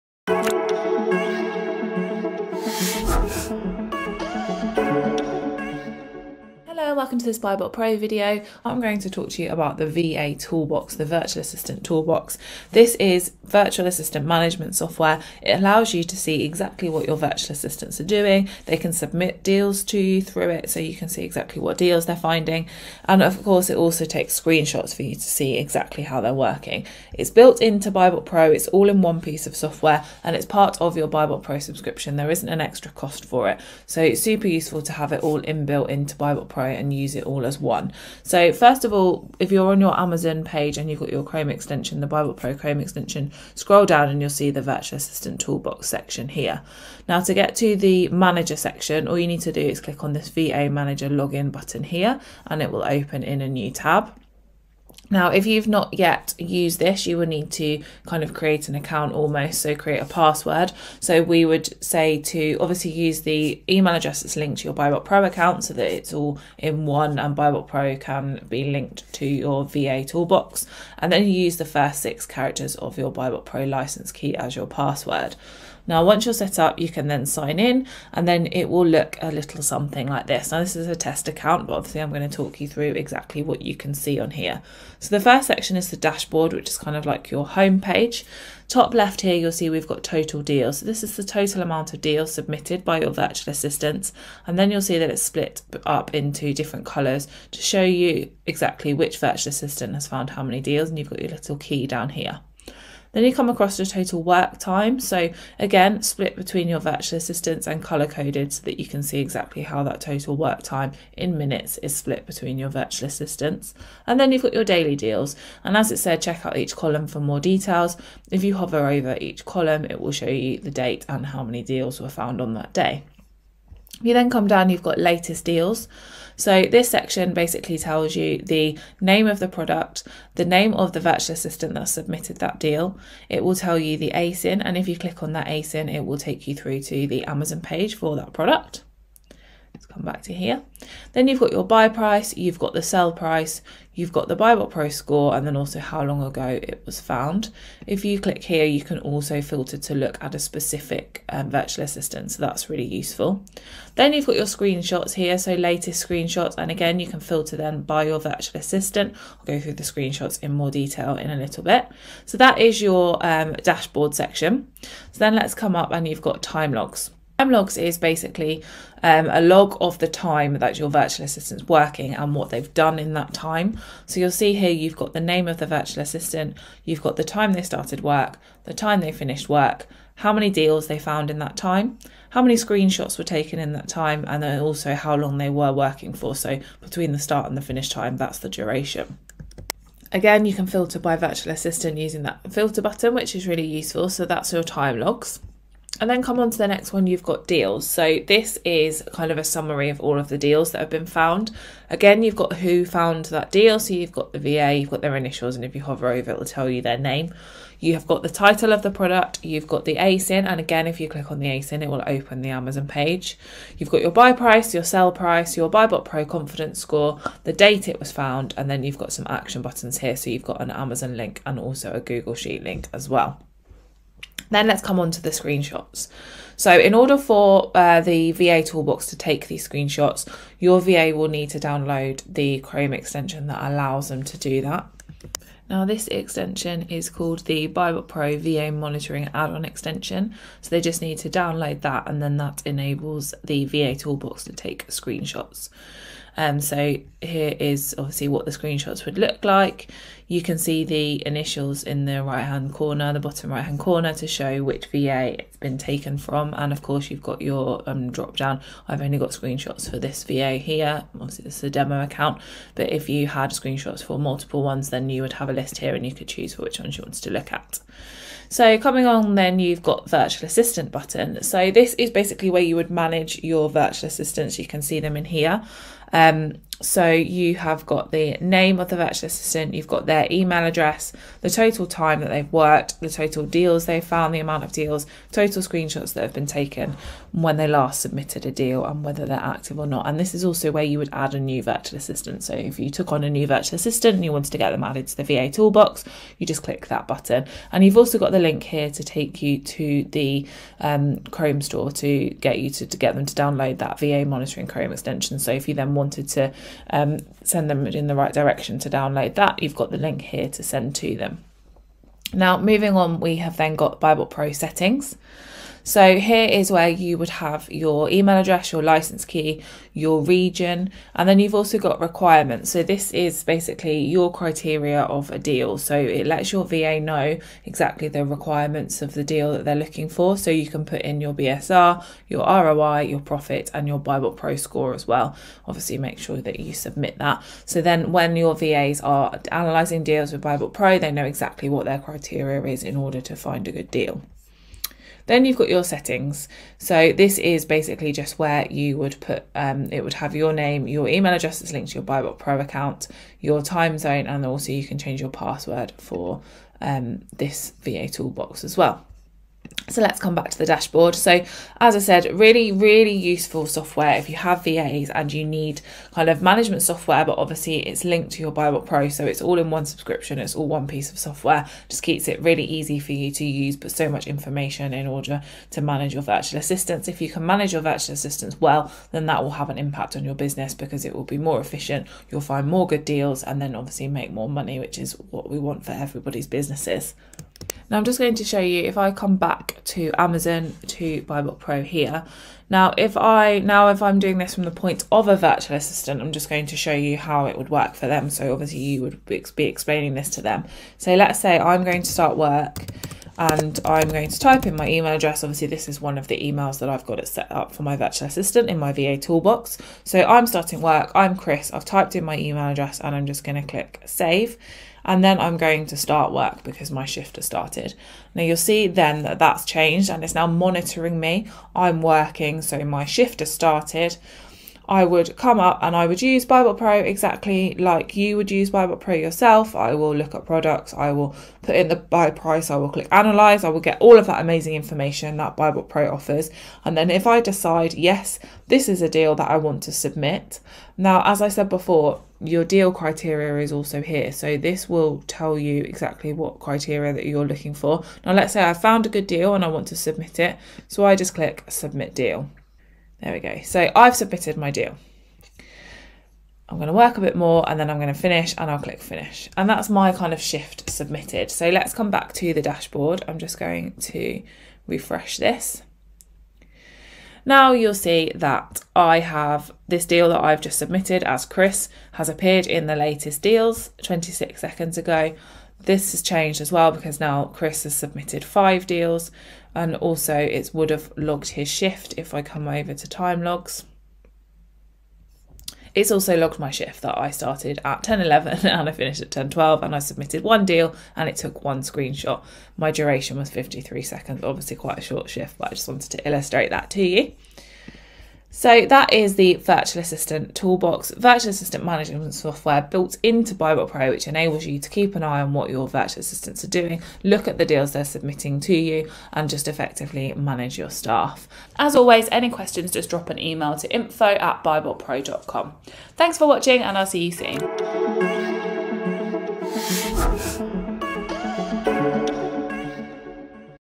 Monitor Your Virtual Assistants With BuyBotPro's VA Toolbox - OA Deals, Time Tracking and Much More!
Monitor your Virtual Assistants with time tracking, screenshots, deal database and graphs so you know exactly what is happening at all times!

How does it work? Watch to find out...